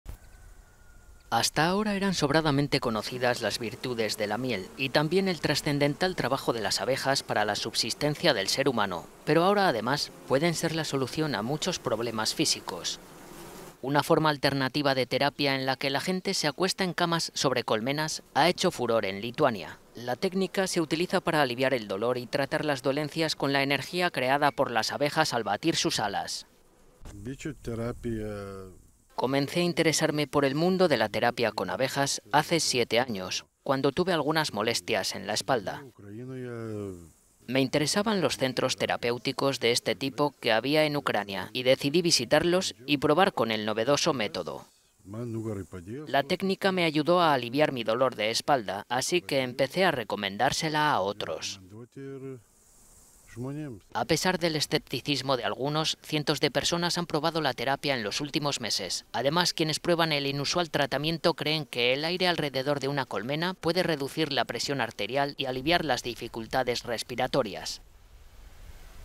El sorprendente poder curativo de las abejas
Una forma alternativa de terapia en la que la gente se acuesta en camas sobre colmenas ha hecho furor en Lituania. La técnica se utiliza para aliviar el dolor y tratar las dolencias con la energía creada por las abejas al batir sus alas…